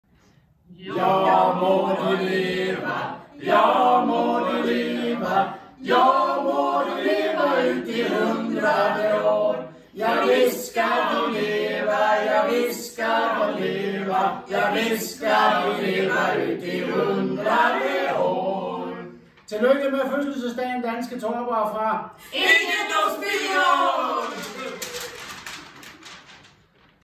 40-års jubilæumshilsen fra Egendomsbyrån v. Jørgen Johansen, ejer og fastighetsmäklare
Egendomsbyrån ønsker Danske Torpare tillykke med 40-års jubilæet og takker for et godt samarbejde.